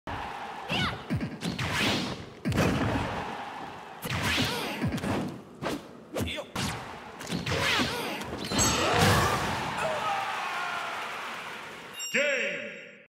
How to read, in three simple steps
Fairly simple way to explain a way to read an opponent, though it can often times go deeper than this the higher you go in levels of play, this is where you should start, since this is effectively the thought process you need to take. The thing is, never take these things as "guaranteed"
A read is just a prediction made by you either based on an opponent's habits, or based on whether or not they do the same things in the same situation. It's up to both players to think a few steps ahead, be it defensively or offensively